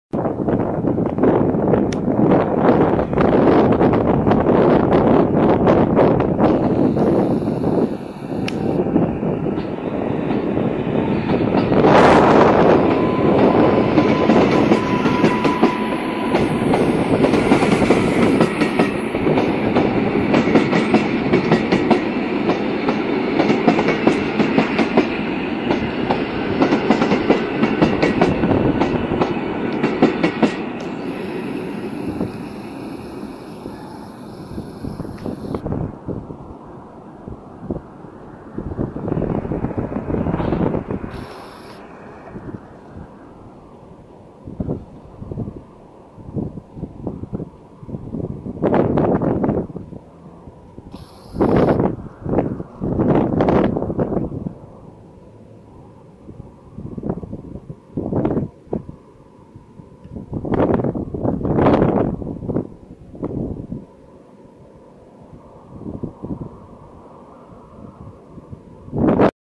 EN57AKM-1641+EN57AKM-1646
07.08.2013. Chińczyki odjeżdżają ze stacji początkowej jako opóźniony o 10 minut pociąg osobowy KM 724 do Warszawy Wschodniej. Opóźnienie składu było spowodowane przepuszczeniem pociągu osobowego KM 725 z Pilawy do Skierniewic z EN57-1665+EN57-1929 w składzie.